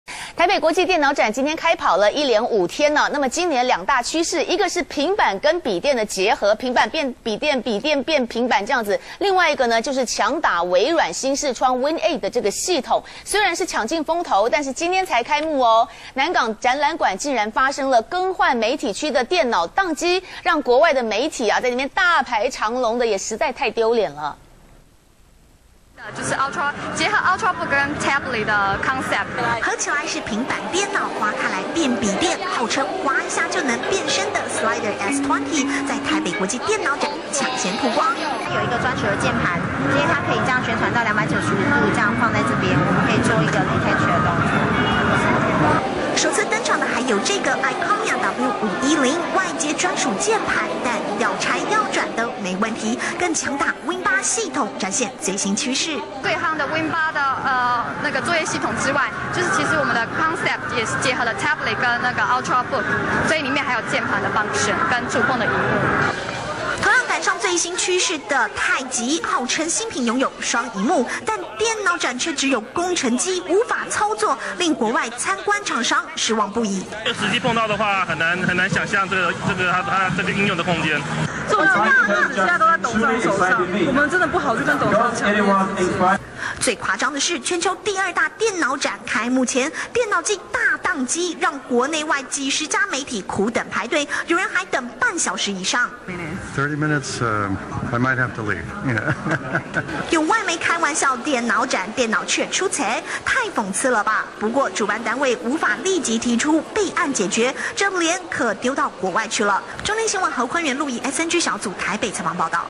電腦展首日 換證區電腦當機外媒乾等
中天新聞》台北國際電腦展5日開跑，今年的熱門趨勢是平版電腦結合筆電，和強打微軟新視窗Windows 8系統搶盡鋒頭。首日開幕，南港展覽館更換媒體證件區的電腦卻當機，讓許多國外媒體在門外大排長龍。